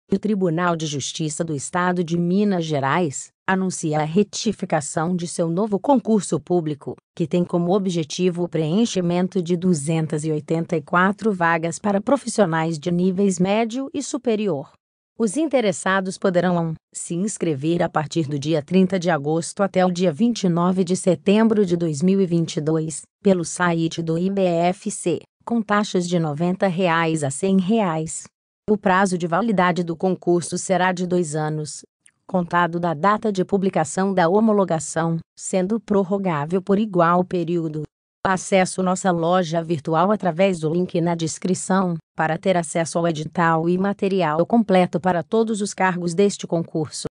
Apostila TJ MG 2022 Oficial Judiciário Assistente Técnico de Controle Financeiro
O Tribunal de Justiça do Estado de Minas Gerais anuncia a retificação de seu novo Concurso Público, que tem como objetivo o preenchimento de 284 vagas para profissionais de níveis médio e superior.

Os interessados poderão se inscrever a partir do dia 30 de agosto até o dia 29 de setembro de 2022, pelo site do IBFC, com taxas de R$90,00 a R$100,00.

➔ Cadastre-se para receber atualizações de todos os concursos 2022. Concurso  TJ MG 2022 👇